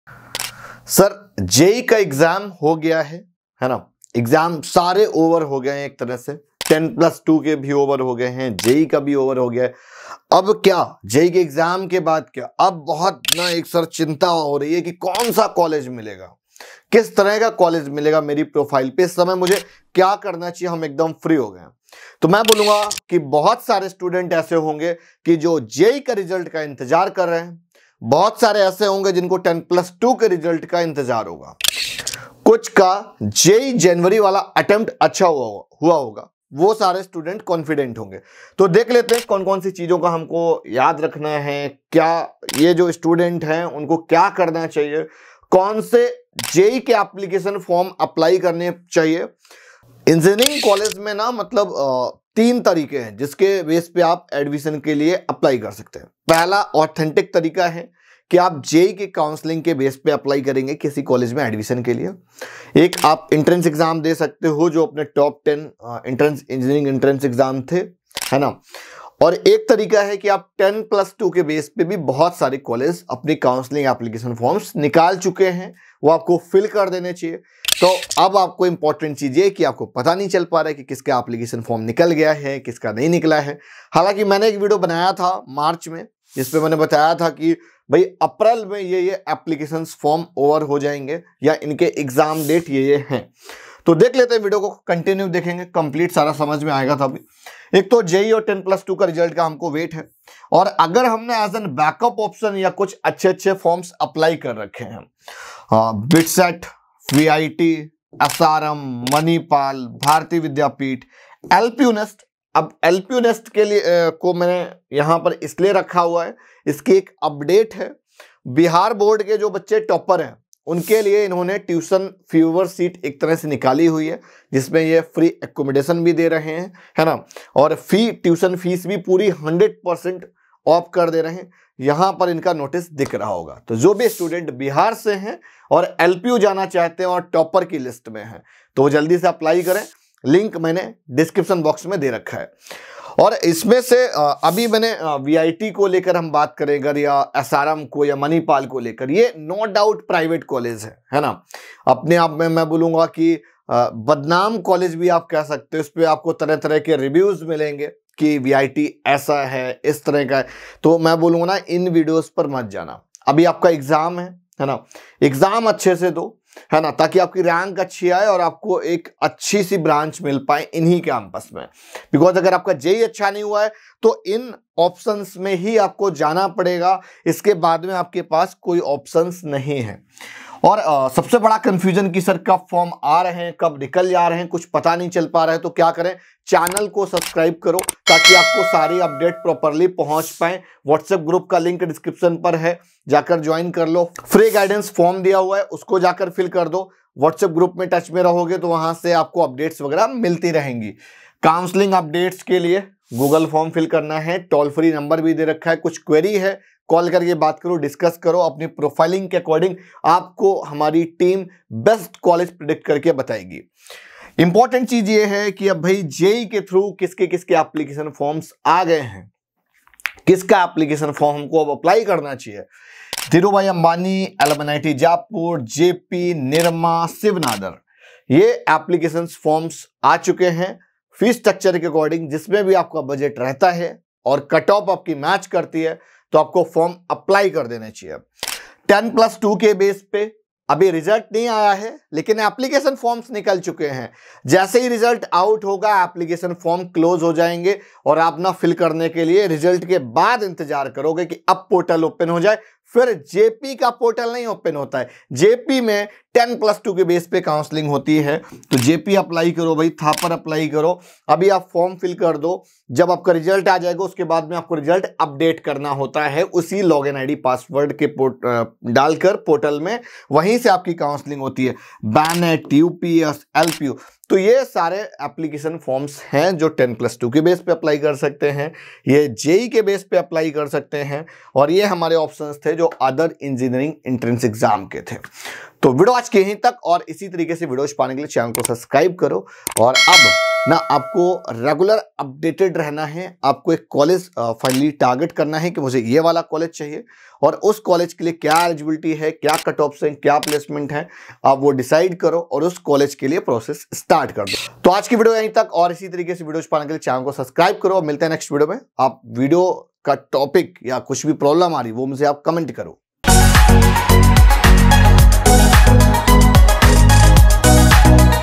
What to do After JEE Mains 2023? | Top Engineering Entrance Exam After JEE Mains
What After JEE Mains 2023? | Top Engineering Entrance Exam After JEE Mains

✅Student Helpline Number:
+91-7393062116

✅Careerkick Prep: JEE & NEET FREE Mock Test Application on Google Play

👉👉👉👉 Use Code - CAREERKICK 👈👈👈👈👈

Channel Name - Careerkick JEE